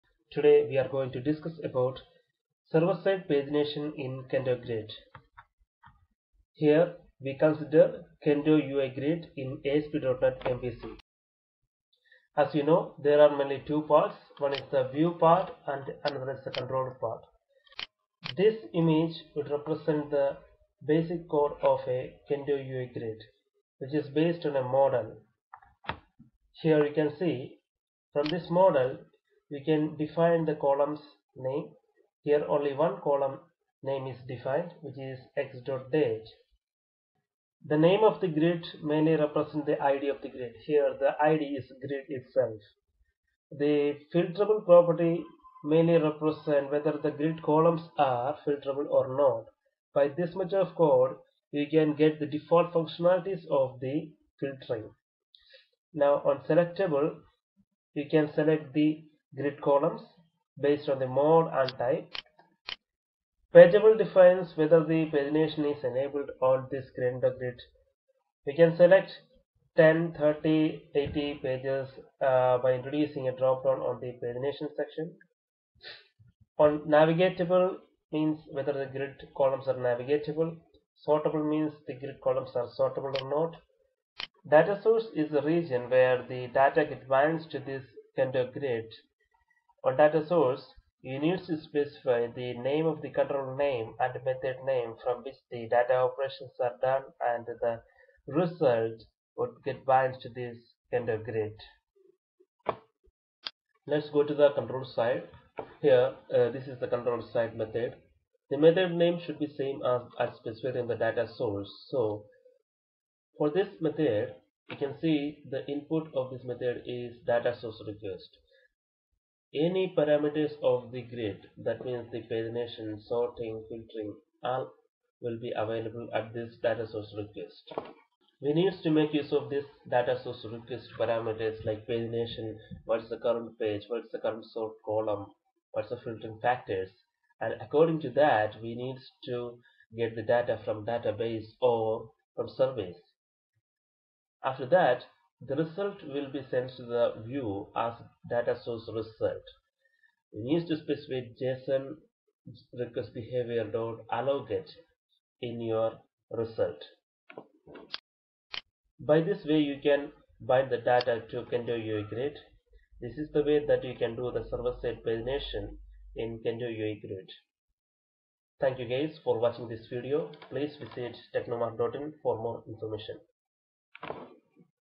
Server Side Pagination in Kendo UI MVC Grid (Dynamically Bind Data To Kendo UI MVC Grid)
Complete steps is described in this video regarding Server Side Pagination in Kendo UI MVC Grid (Dynamically Bind Data To Kendo UI MVC Grid)